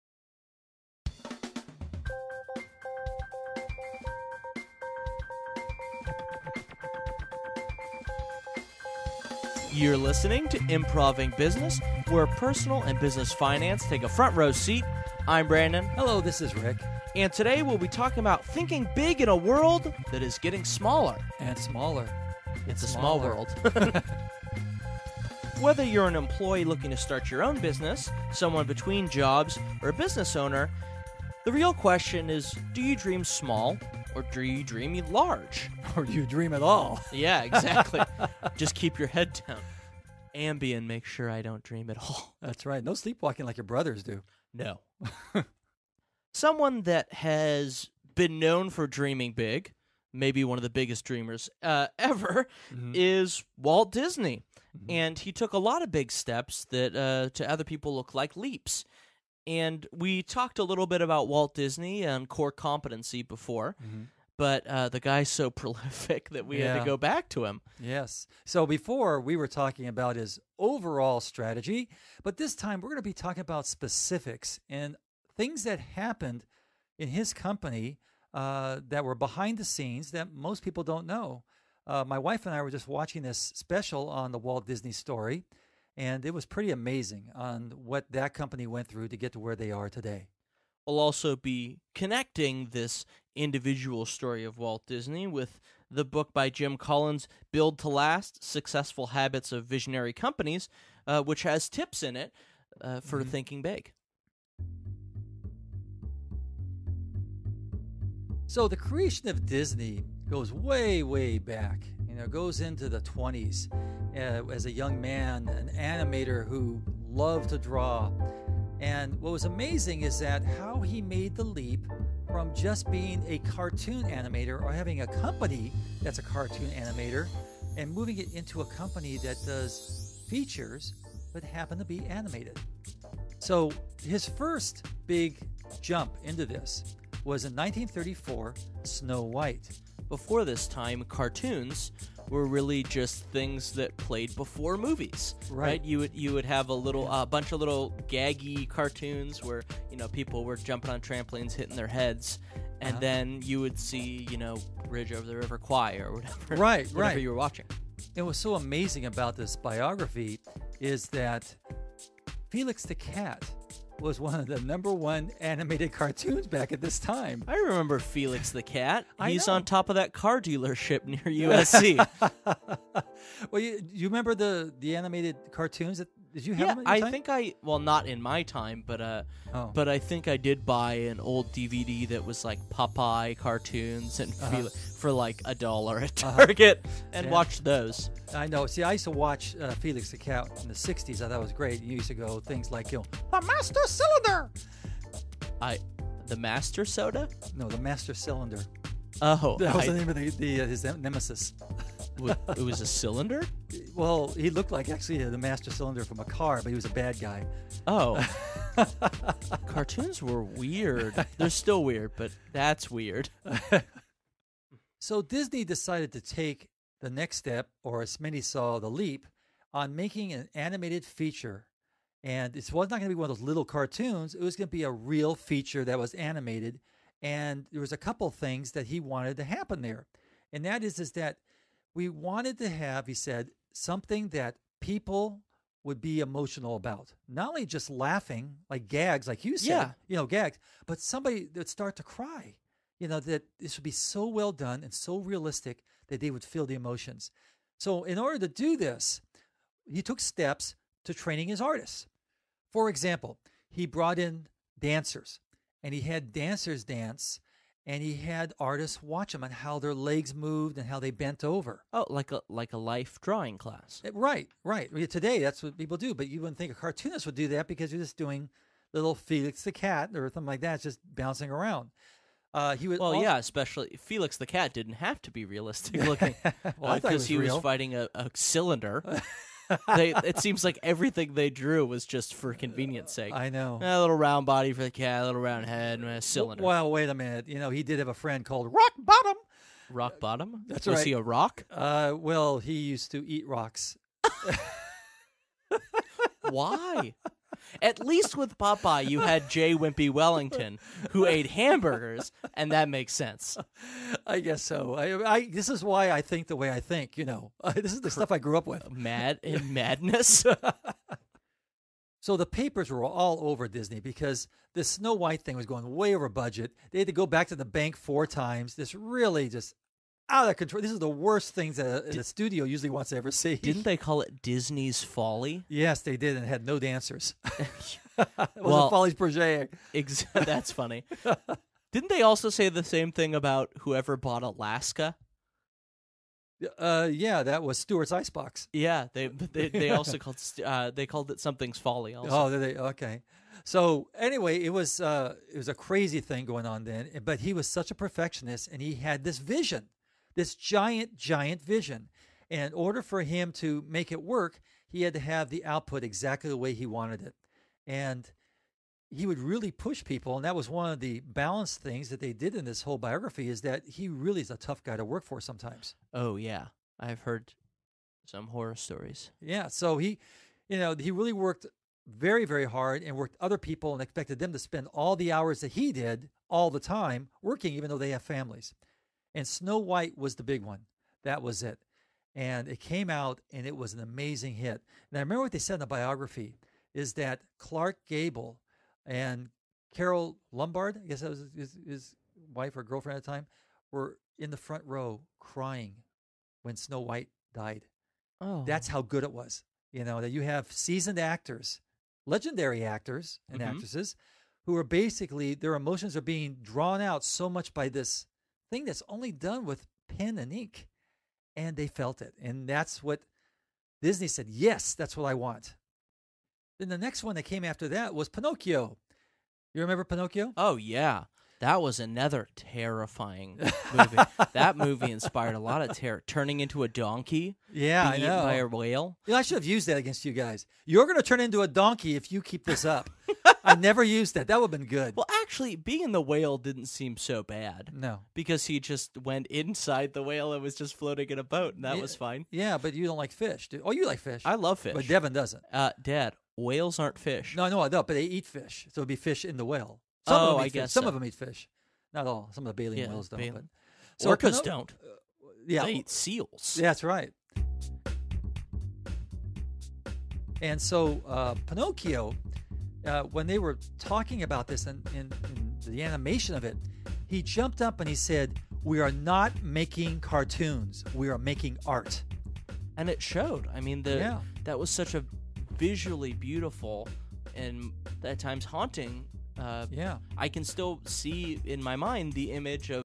Episode 42 Thinking Big Like Walt Disney In An Ever Shrinking World
Click Here To Hear The Full Episode

Rick Norris and Brandon Norris - Improv-ing Business Podcast

    What is your BHAG (Big Hairy Audacious Goal)?
        Can you write is out clearly in 20 words or less?
        If it isn’t clear or is full of industry jargon, rewrite it.
        Does your BHAG copel others to join your vision?
        Does it excite those who are going into business with you?
        Does your BHAG move forward, or just stagnate?

Source: Built to Last, Successful Habits of Visionary Companies, by Jim Collins

Receive Our Podcast In Your Email Inbox!